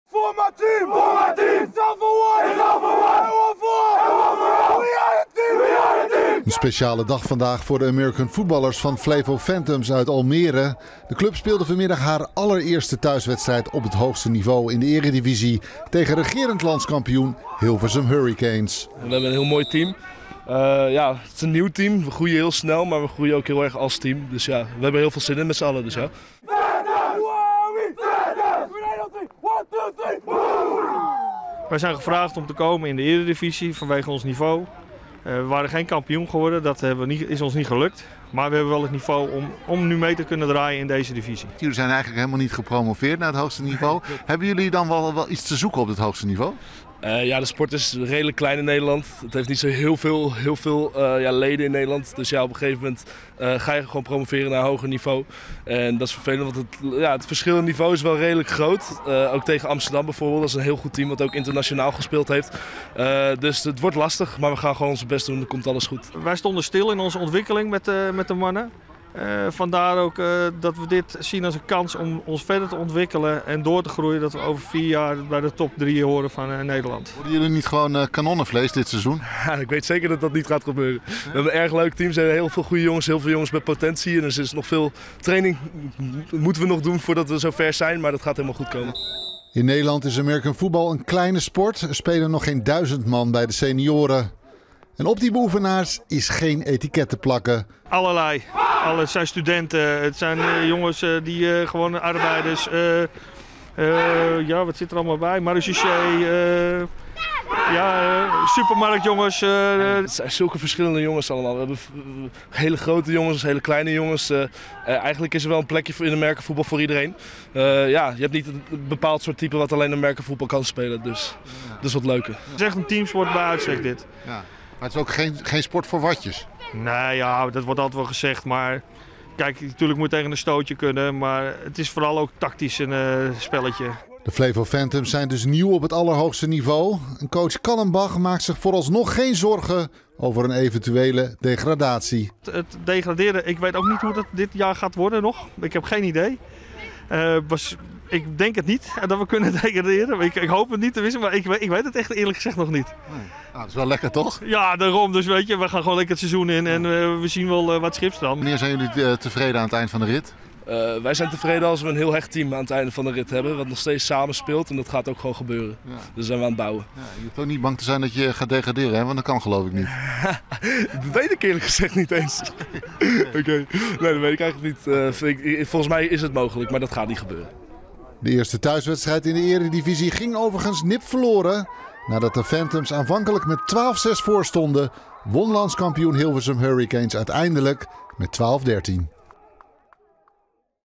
Flevo Phantoms verliezen thuisdebuut in eredivisie
De American Footballers van Flevo Phantoms uit Almere hebben zondagmiddag hun thuisdebuut in de eredivisie nipt verloren.

Regerend landskampioen Hilversum Hurricanes was op sportpark De Marken in Almere Haven met 12-13 nipt te sterk voor de formatie van coach Ronald Callenbach. Aanvankelijk leidden de Phantoms nog met 12-6.

Flevo Phantoms speelt dit seizoen op het hoogste landelijke niveau en behoort hiermee tot de beste 8 teams van Nederland. Het afgelopen seizoen eindigde de ploeg nog als derde in de eerste divisie, maar om de eredivisie sterker te maken werd de ploeg gevraagd om op het hoogste niveau te komen spelen.

Coach Ronald Callenbach ziet dit seizoen als een mooie uitdaging om veel ervaring op te doen. Hij wil met de Flevo Phantoms over 4 jaar bij de Top 3 van Nederland horen.